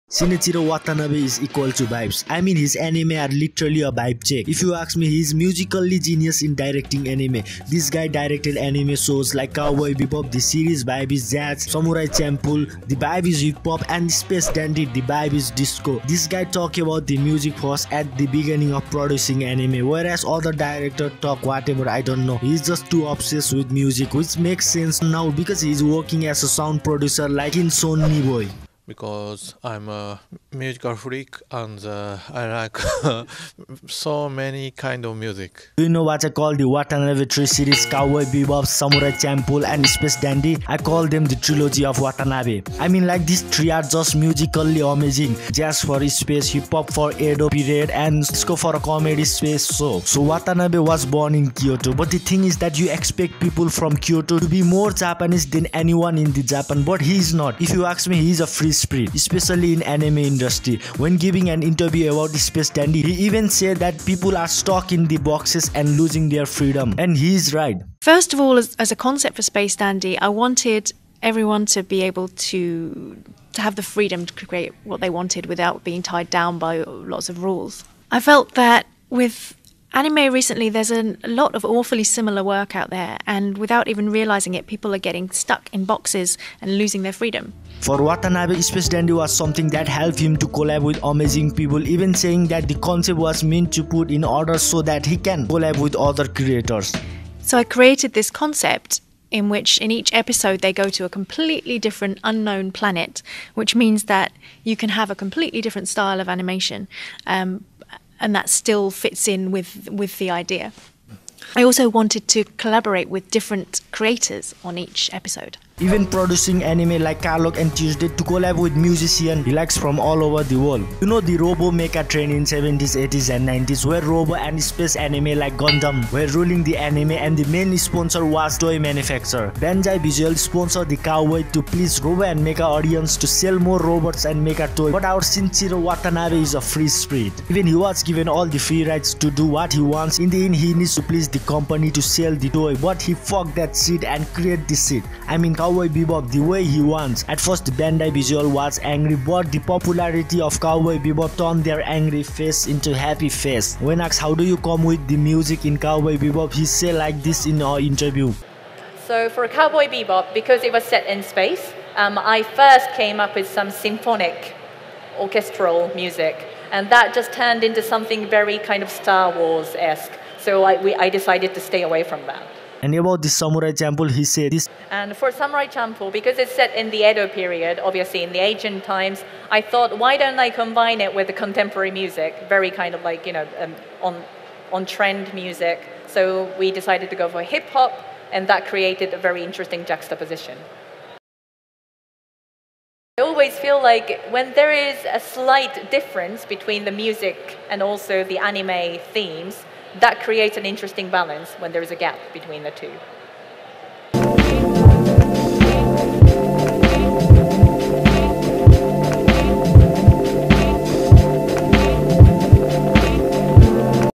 Free Spirit of Anime Industry. Ft:- Shinichirō Watanabe.
The free spirit of the anime industry, the musically Genius Anime Director Shinichirō Watanabe. He has directed shows like cowboy bebop, Space dandy, Samurai Champloo and many more.